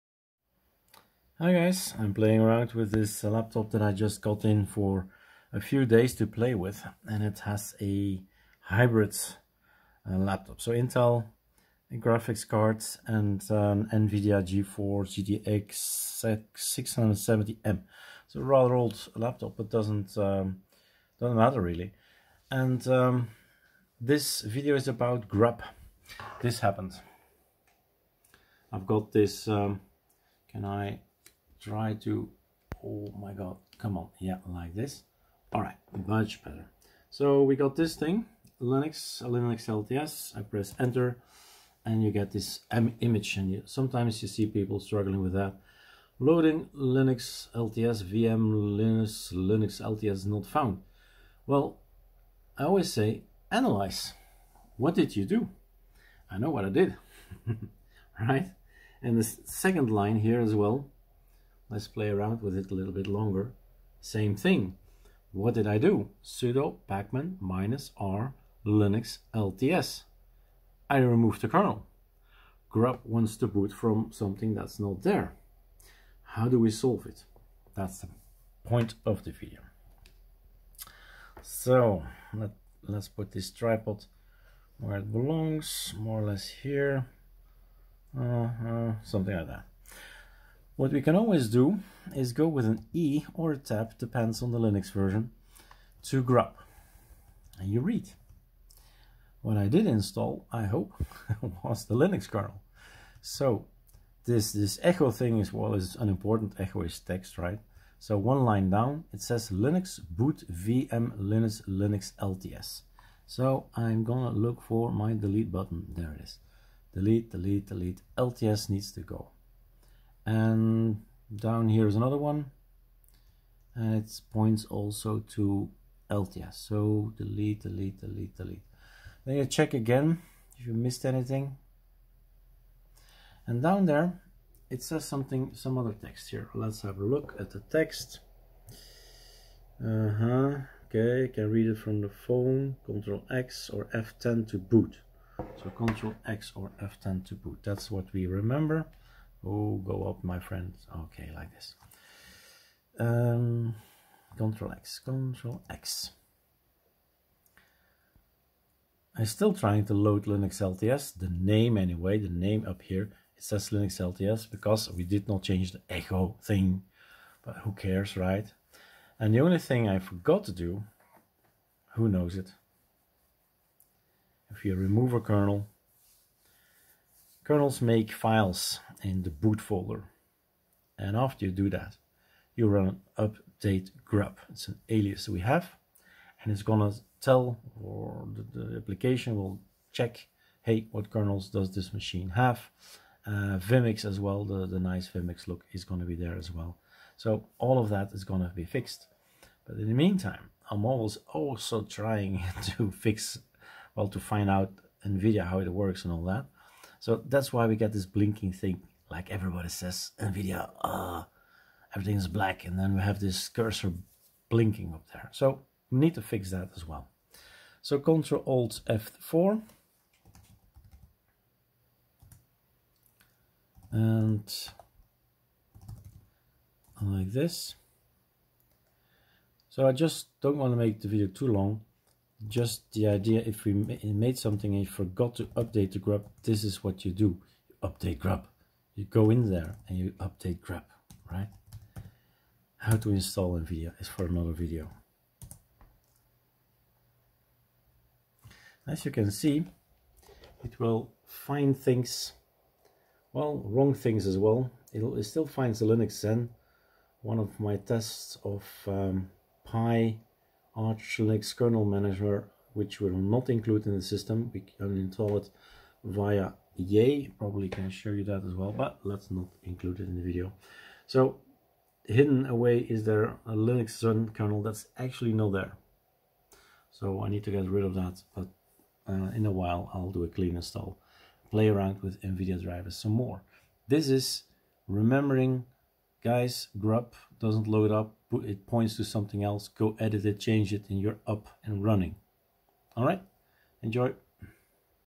ArcoLinux : 1793 Forgot to 'update-grub' - edit grub and boot back into your system
I deleted the Linux-lts kernel and installed the linux kernel but forgot to update-grub